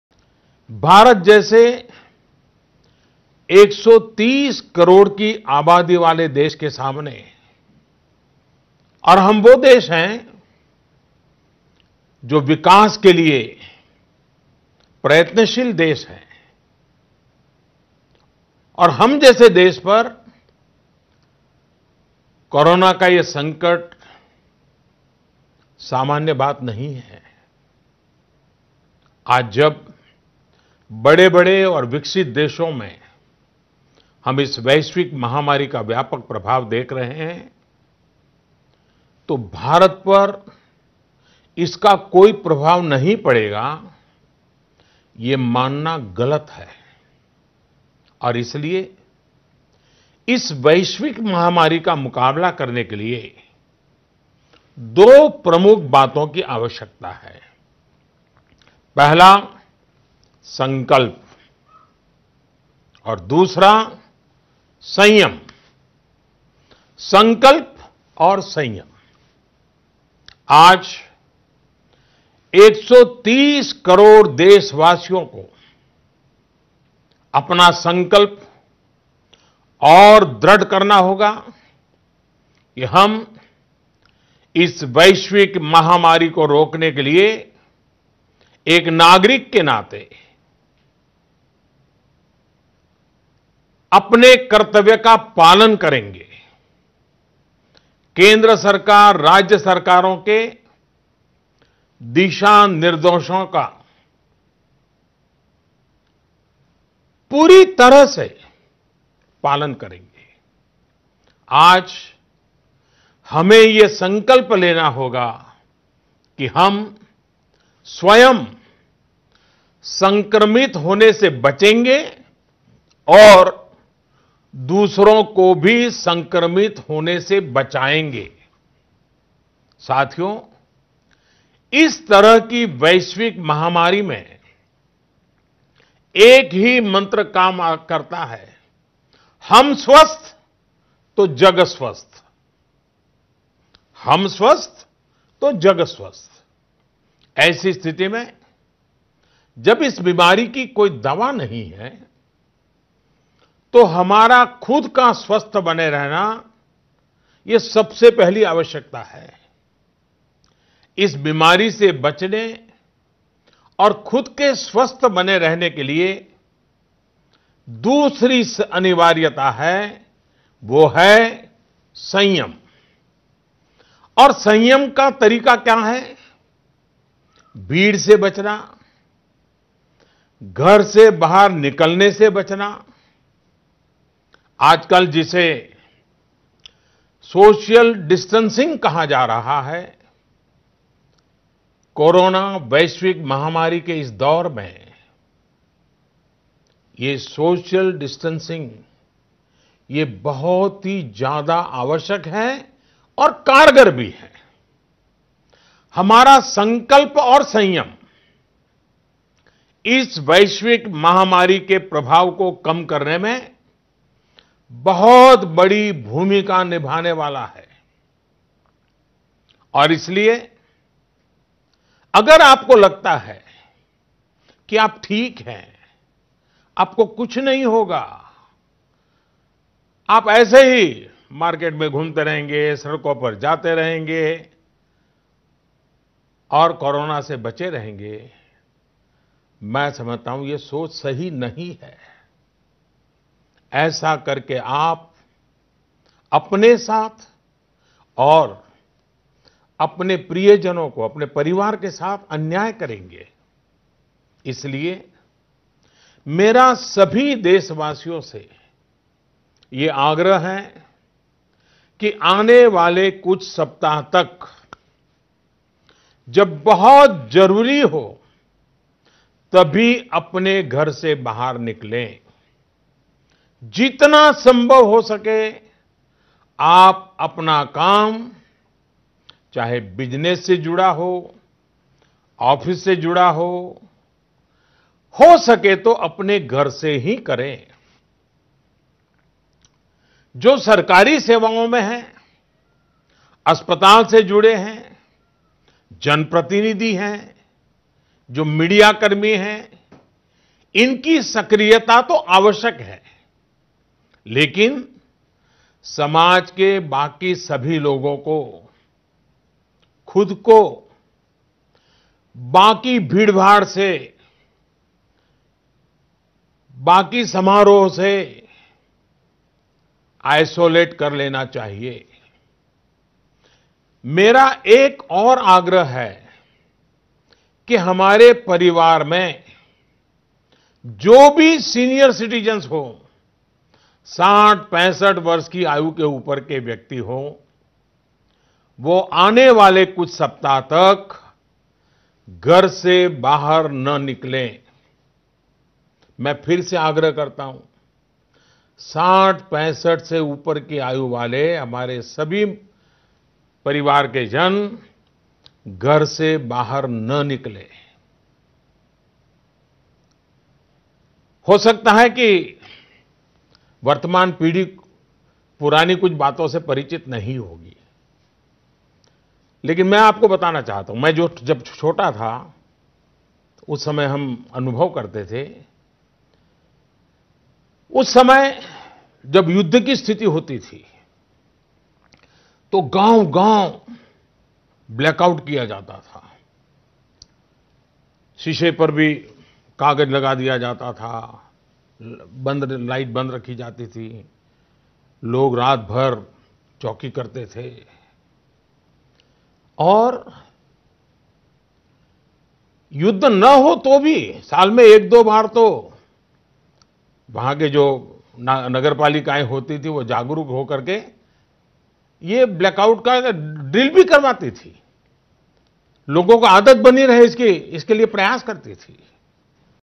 Why is social distancing important to fight Coronavirus? Hear it from PM Modi!
Prime Minister Narendra Modi's address to the nation on Coronavirus and efforts to combat it